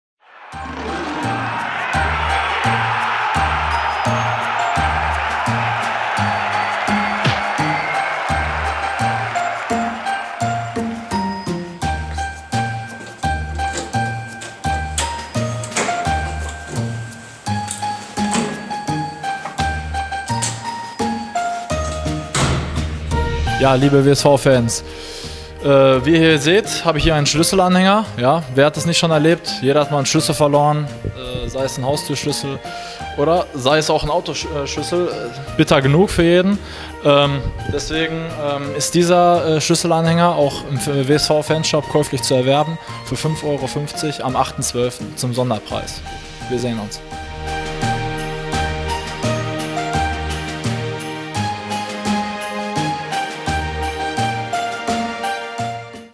WSV-TV: Adventskalender 7. Türchen
WSV-TV präsentiert das 7. Türchen mit Marvin Schurig.

Was für ein Angebot (gültig am 08.12.14) euch erwartet, seht ihr im Video.